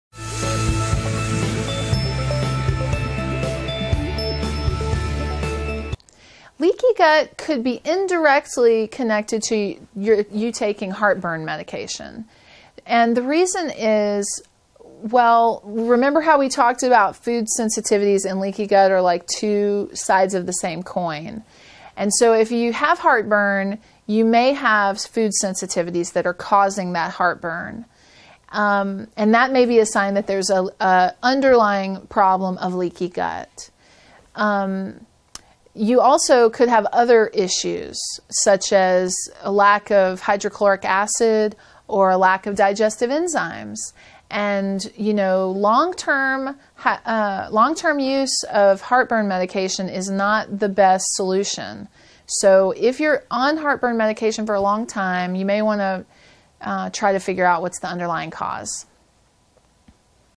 How could LEAKY GUT be connected to my heartburn pills?
Cass Nelson-Dooley, M.S.
Integrative Medicine Writer and Educator
Answers a series of questions about Leaky Gut and the Leaky Gut Screen

Get the test LeakyGutScreen.com